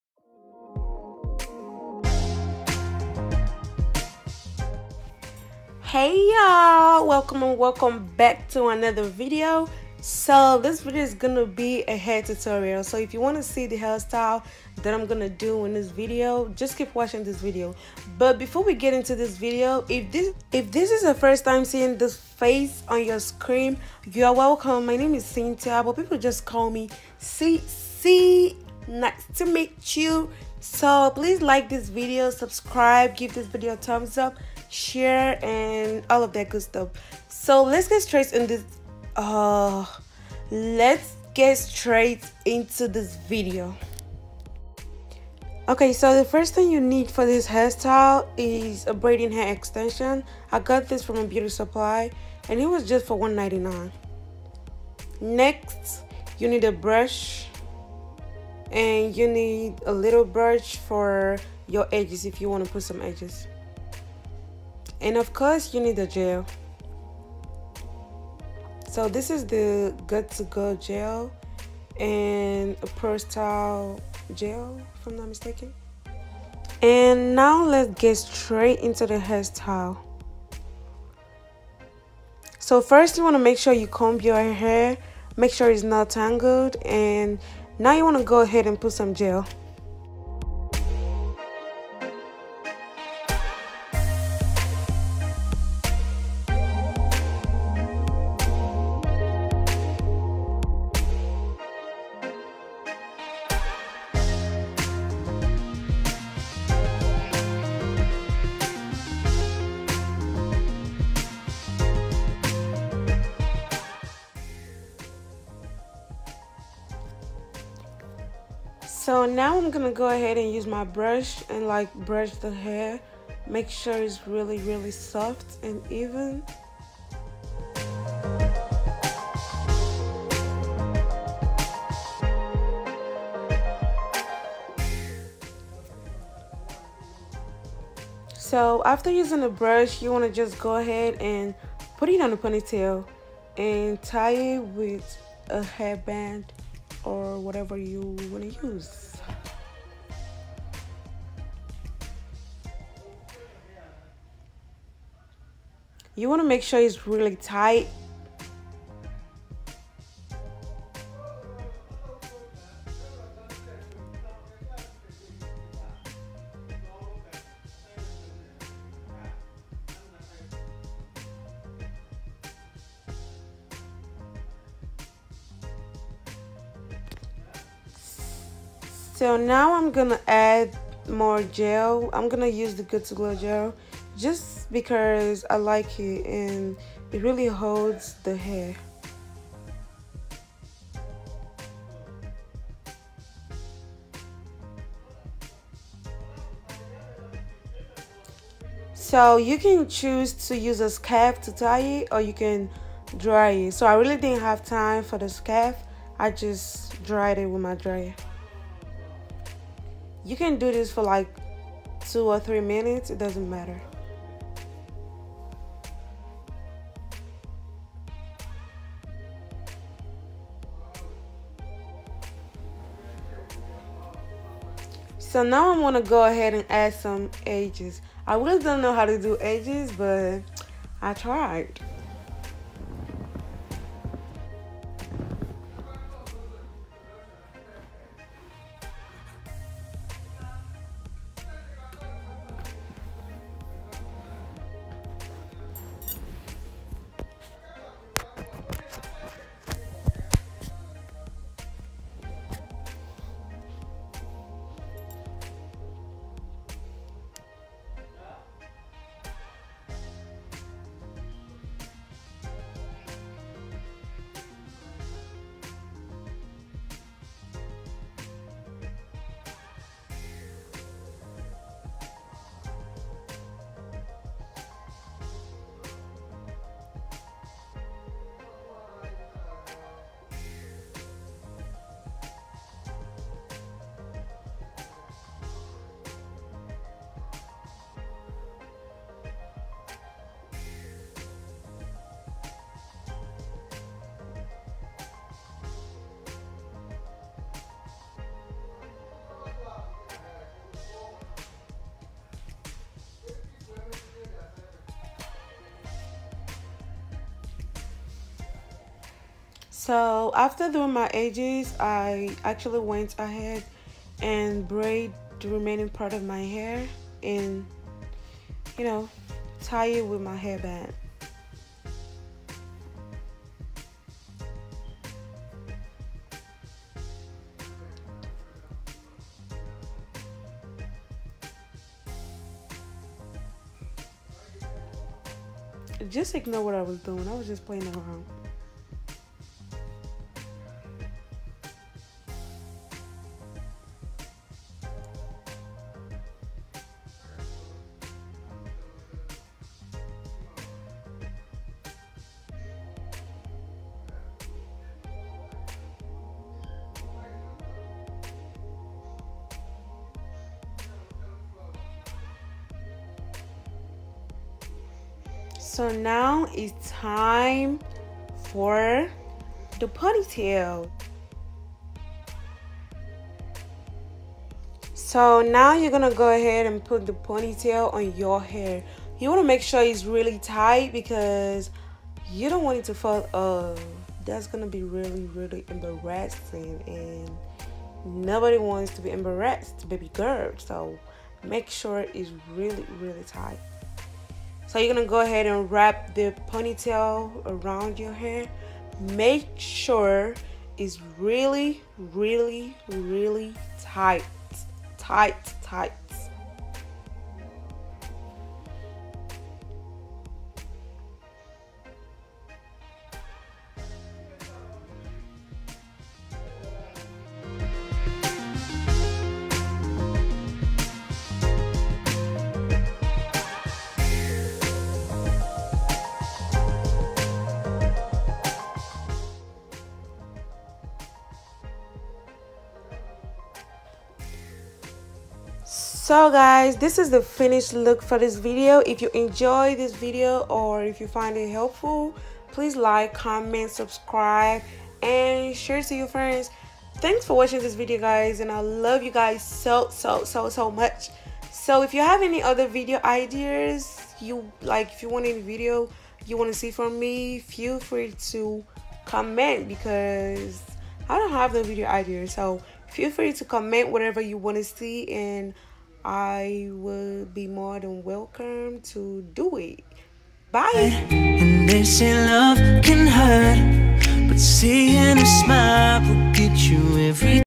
HOW TO: QUICK & EASY TOP KNOT BUN/BRAIDING HAIR| STEP BY STEP.
Hey y’all!!! 😍😍

  In this video, I showed y’all how you can make your top knot bun in less than 30 minutes using a braiding hair extension.
I LOVE Y’ALL SOOOOOOOO MUCH!
Feel  free to stalk me!!!
SC: babyjunior222